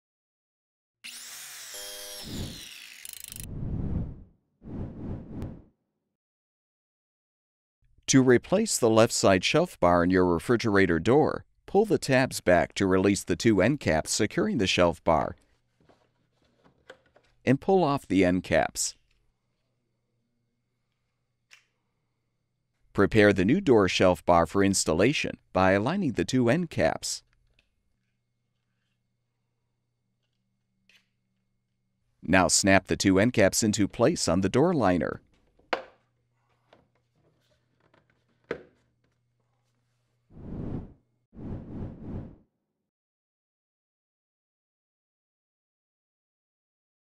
Whirlpool Matrix Refrigerator Left Door Shelf Bar Replacement W11106290
This video provides step-by-step repair instructions for replacing the left door shelf bar on a Whirlpool Matrix refrigerator (Model WRT318FMDB02). The most common reason for replacing this part is if it is damaged or missing.

Whirlpool, Maytag, Amana, Kitchenaid, Kenmore